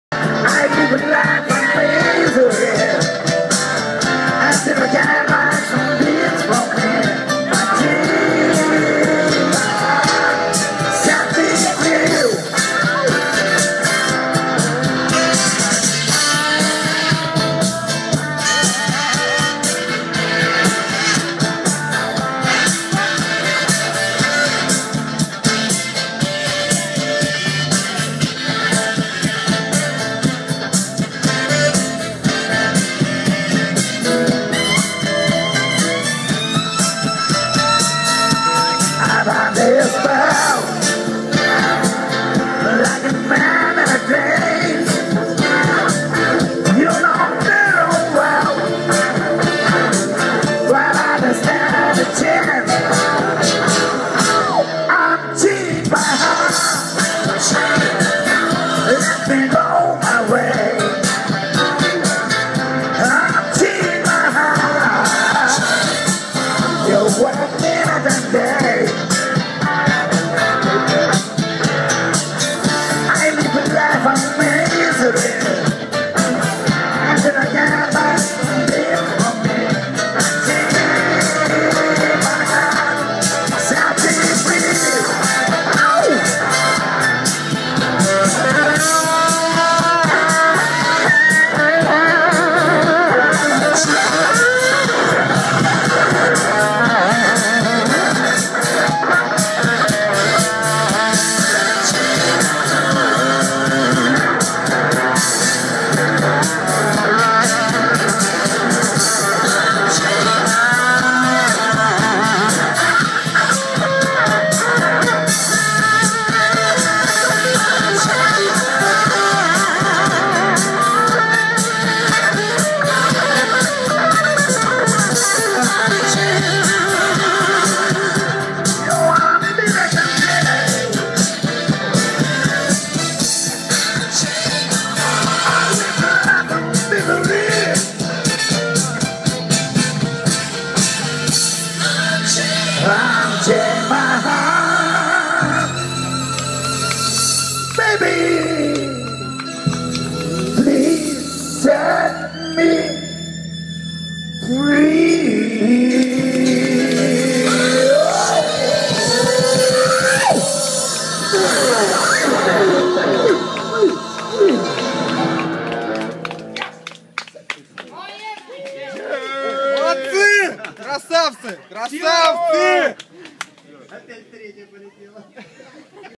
Танец победителей
Победители Сибирской регаты 2013
4 августа 2013 г. я/к "Наука"